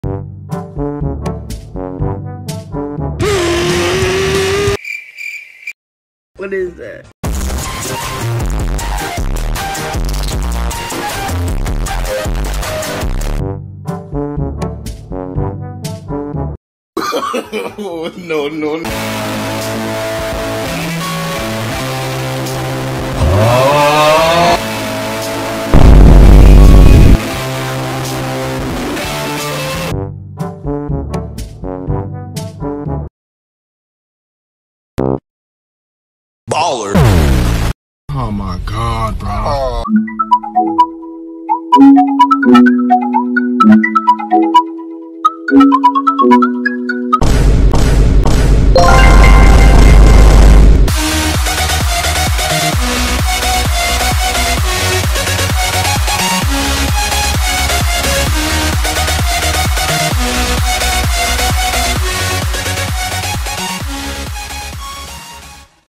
AI GENERATES ROBLOX TDS TOWERS! (MEMES)
(made this cuz im bored)
we're evolving. just backwards...
btw art generated in DALL E 2